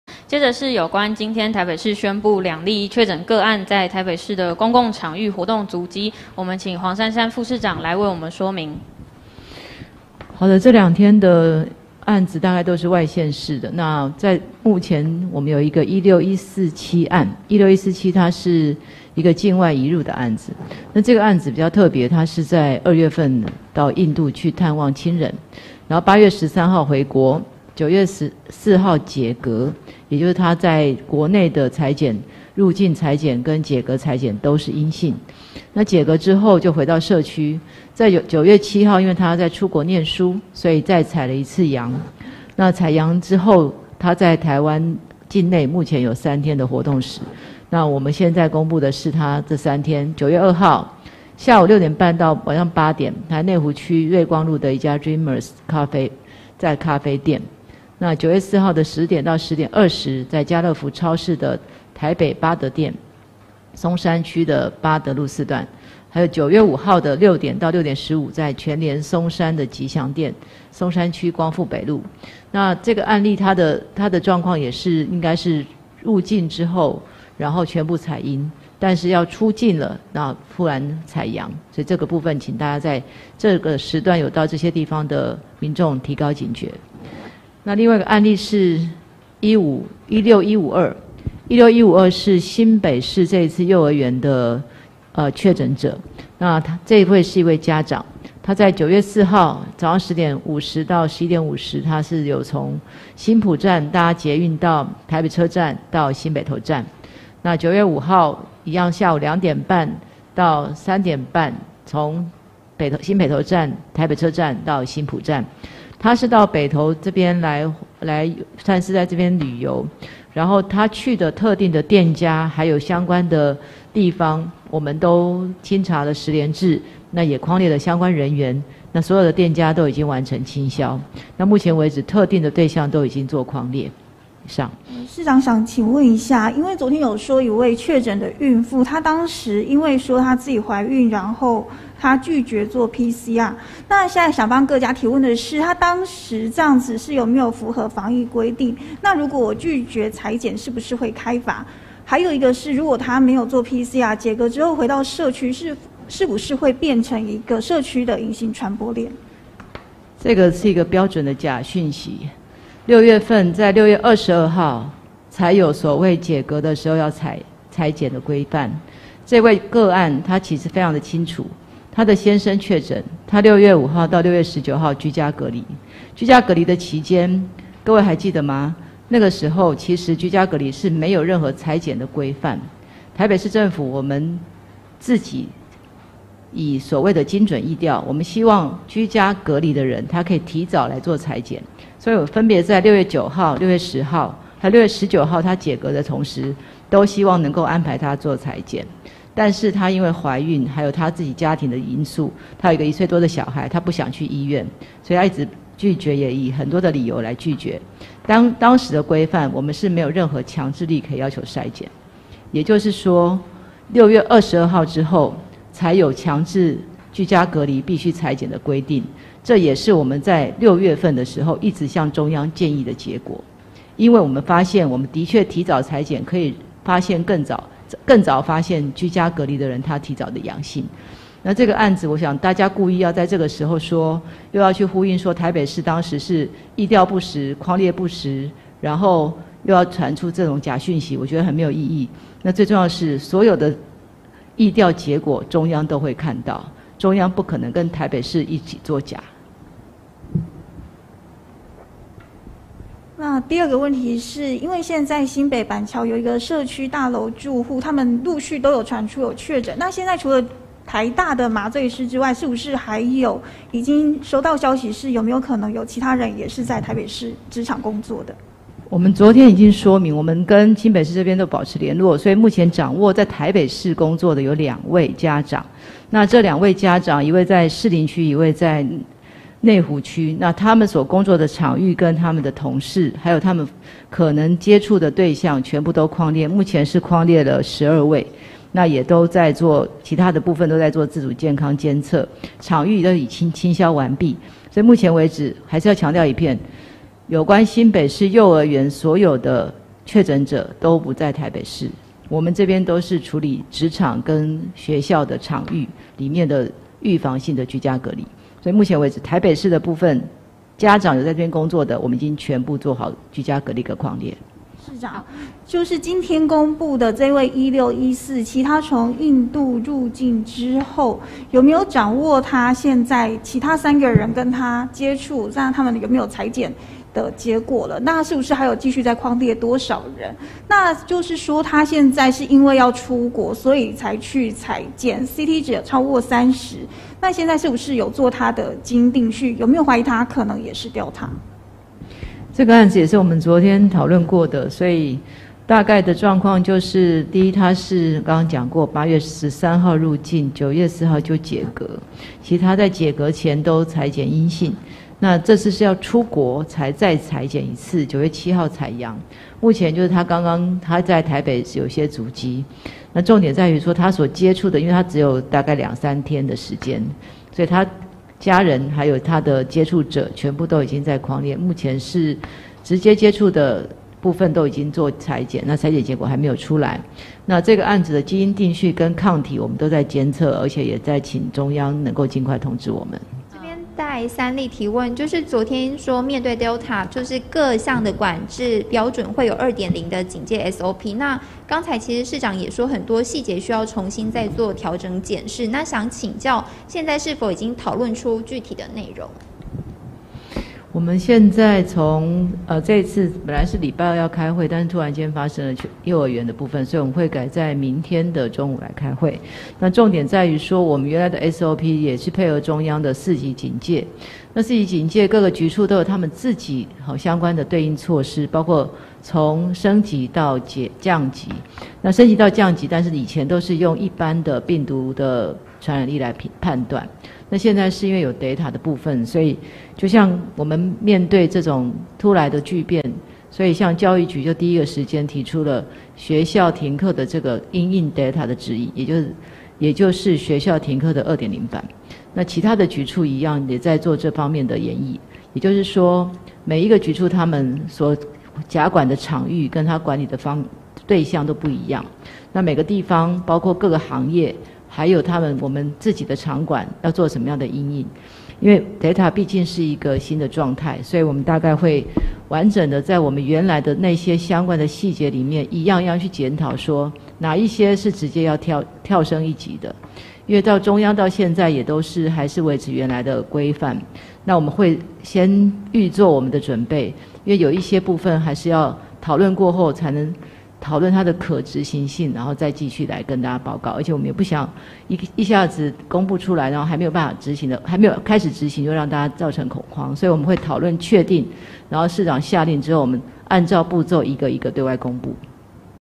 【0908精華必看】Delta來勢洶洶 北市整備資源嚴正以待｜台北市流行疫情指揮中心記者會
【記者會精華】
0:00 公共場域足跡（案16147、案16152）

【媒體提問】
2:15 確診孕婦居隔期間拒作採檢是否符合防疫規定？
4:14 板橋社區大樓住戶確診，是否有接到職場在台北的個案？
5:18 案16147為境外移入個案是否有進行基因定序？
6:24 面對delta病毒是否已經制定相關SOP？